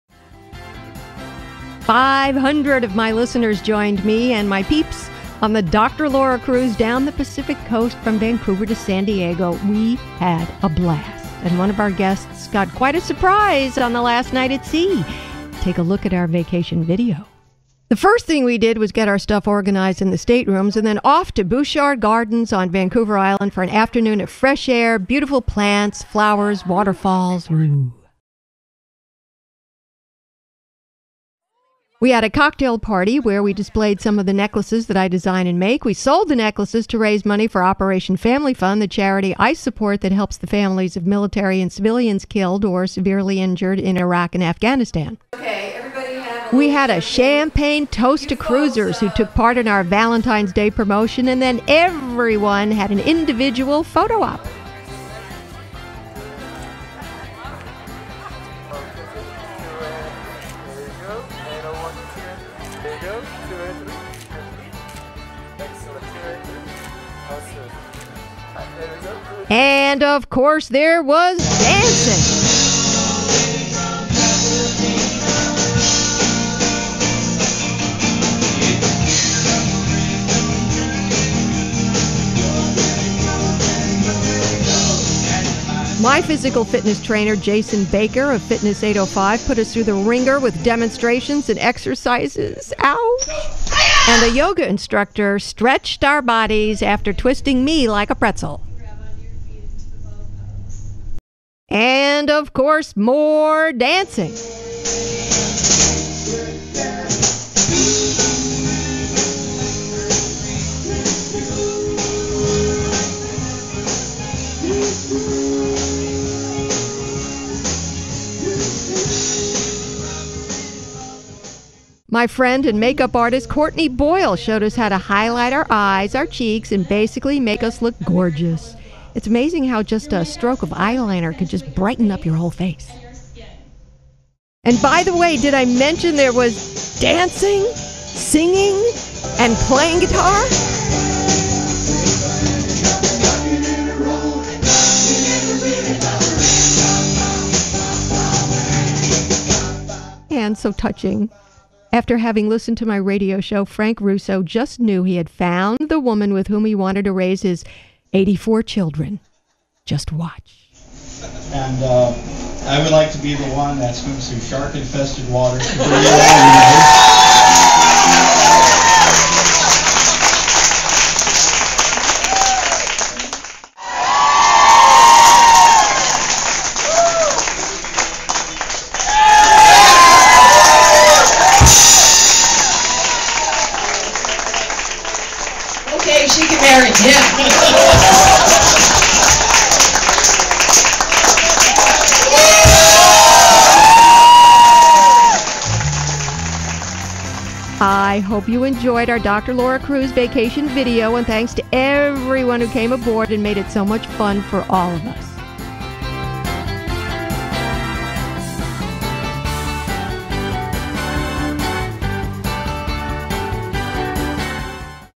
The Dr Laura Cruise 2008
Highlights of the activities, adventures and fun that occurred on the Dr. Laura Cruise 2008.